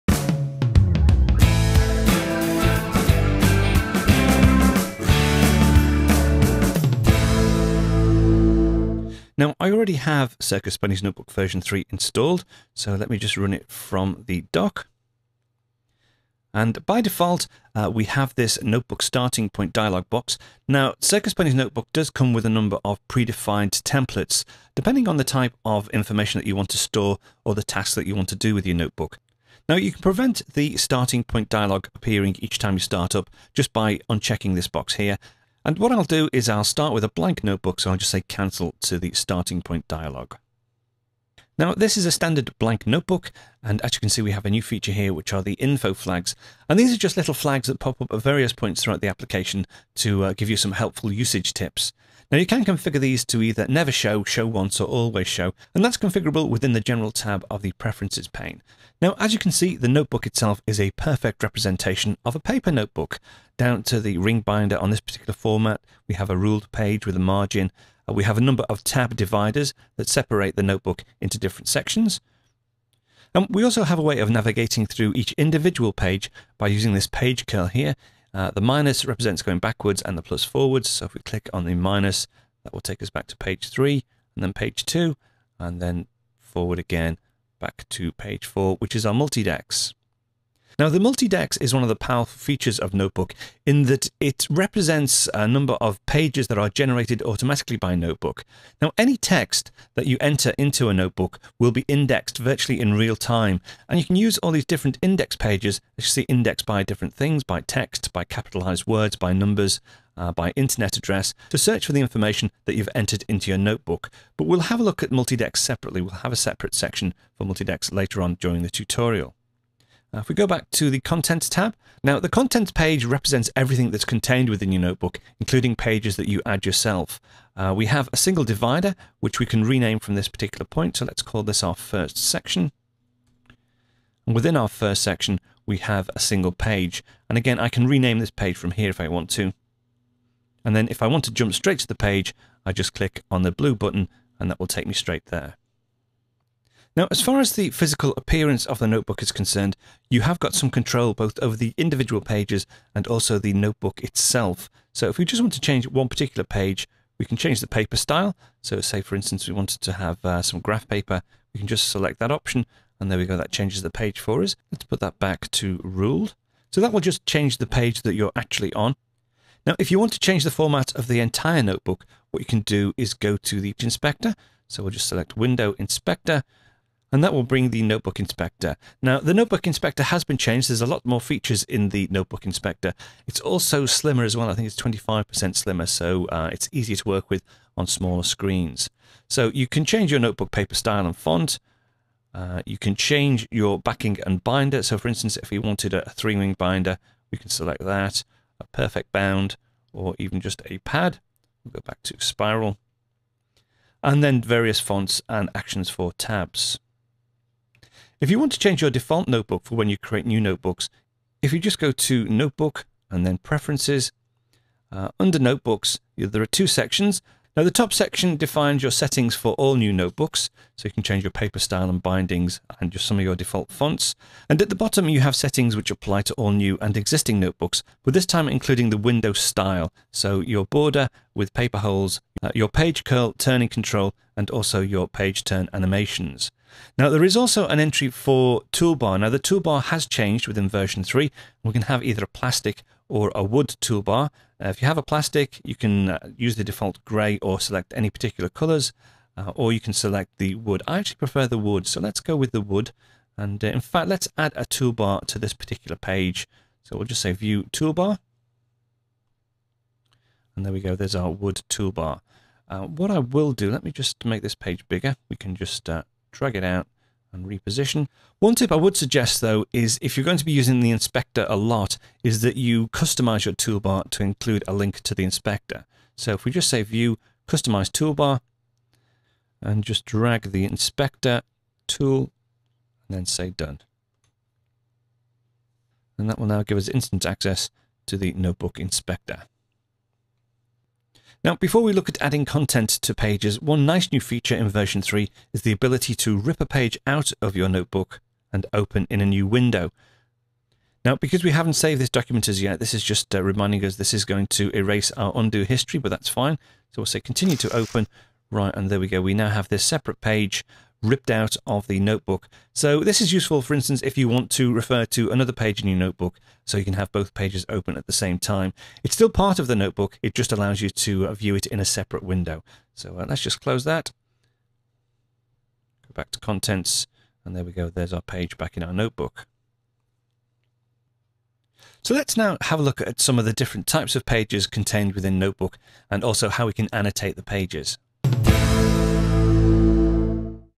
Stay Super Organized Using NoteBook
Circus Ponies NoteBook's electronic notebooks are perfect for storing notes, images, documents, to dos, movies. Create outlines and lists, choose from different paper styles and Notebook templates.